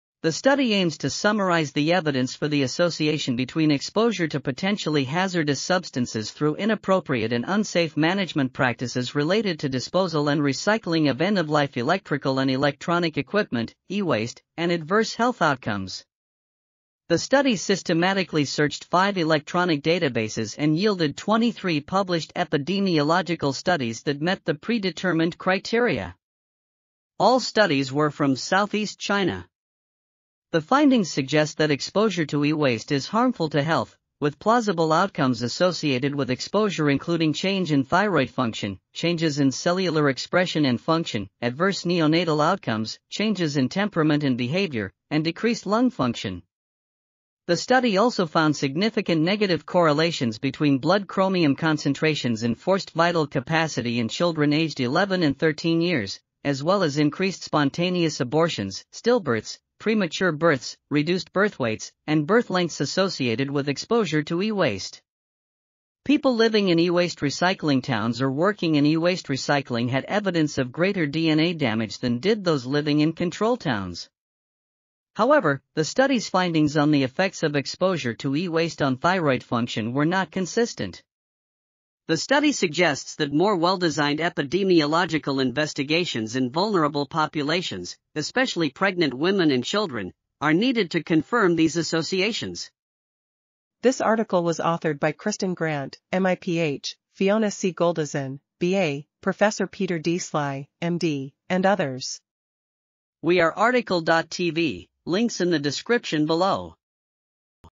Health consequences of exposure to e-waste: a systematic review | RTCL.TV
### Keywords ###

### Article Attribution ###
Title: Health consequences of exposure to e-waste: a systematic review
Authors: Kristen Grant, MIPH, Fiona C Goldizen, BA, Prof. Peter D Sly, MD, Marie-Noel Brune, MSc, Maria Neira, MD, Prof. Martin van den Berg, PhD ,and Rosana E Norman, PhD
Publisher: Elsevier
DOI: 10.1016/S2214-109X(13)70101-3

### Video Timestamps ###
0:00:00 - Summary
0:01:33 - Title
0:01:43 - Tail
0:01:47 - End